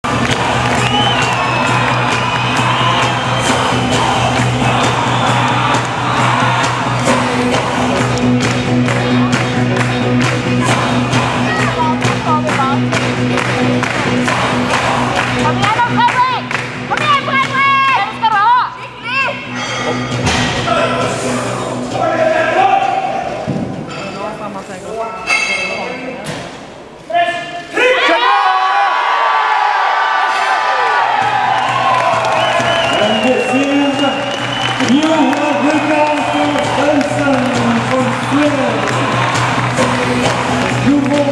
Fredrik Svensson - 361kg Benchpress World Record @ WEC 2013
361kg world record